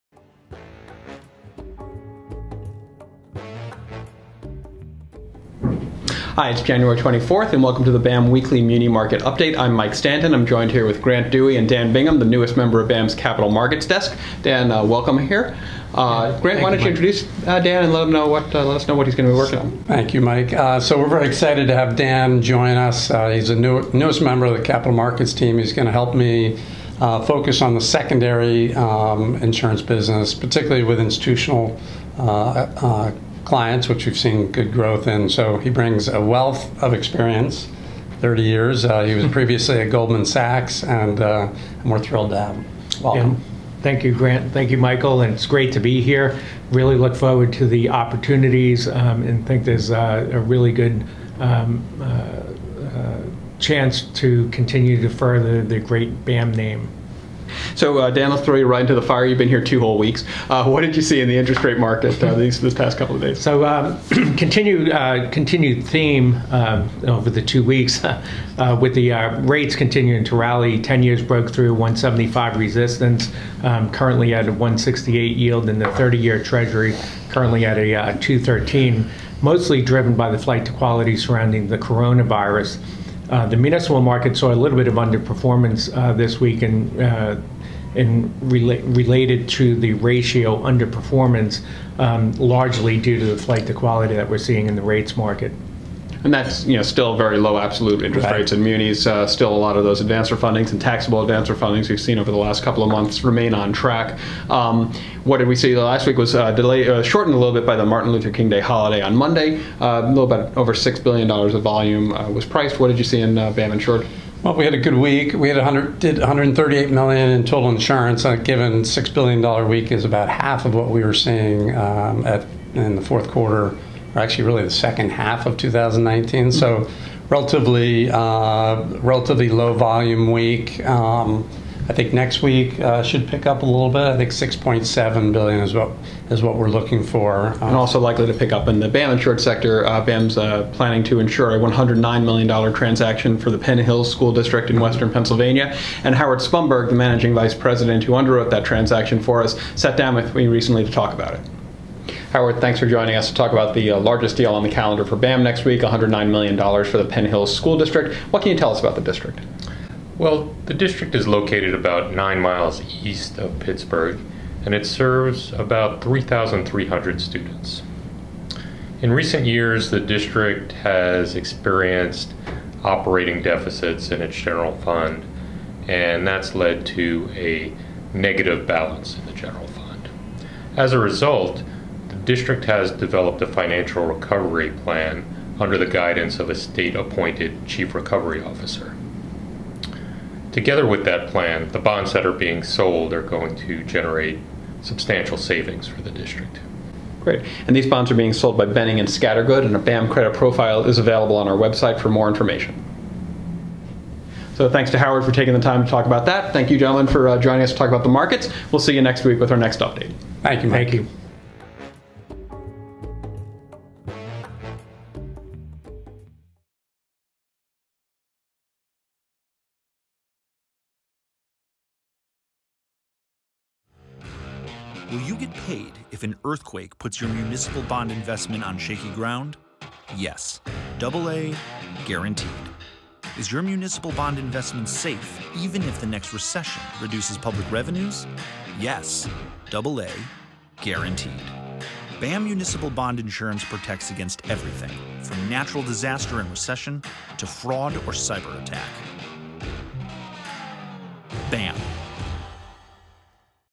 BAM Muni Market Update - January 24, 2020
Municipal market yields underperformed a global rally in Treasuries that was sparked by concerns about the spread of the coronavirus within China. Looking ahead, BAM is preparing to insure a $109 million issue for the Penn Hills School District in Western Pennsylvania.